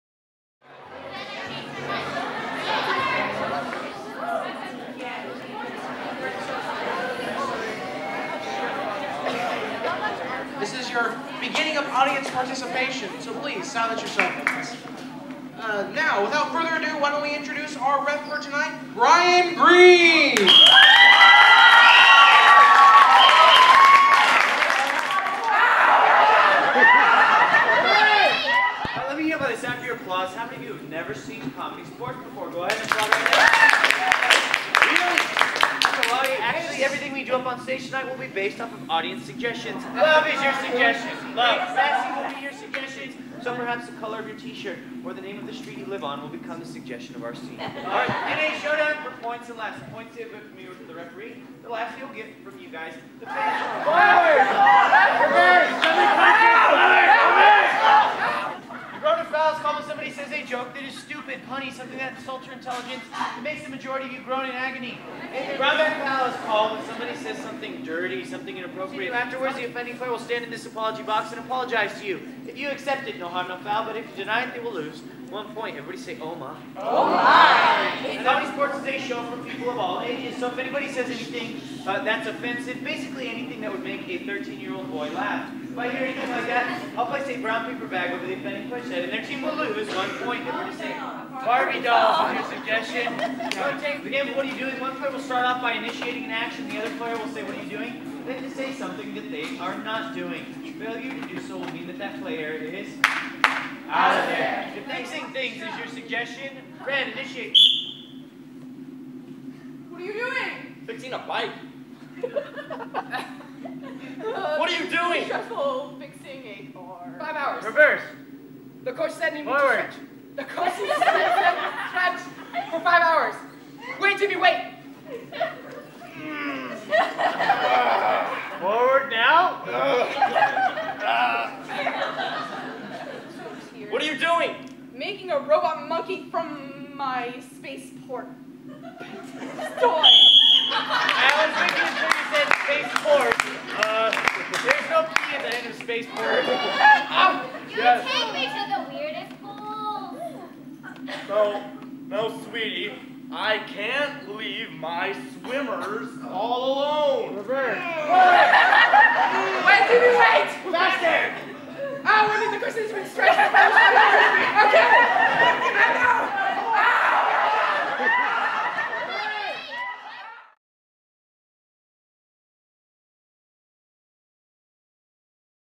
Comedy Sportz: An Introduction
A documentary giving a brief summary of Comedy Sportz, an Improv based sketch comedy show.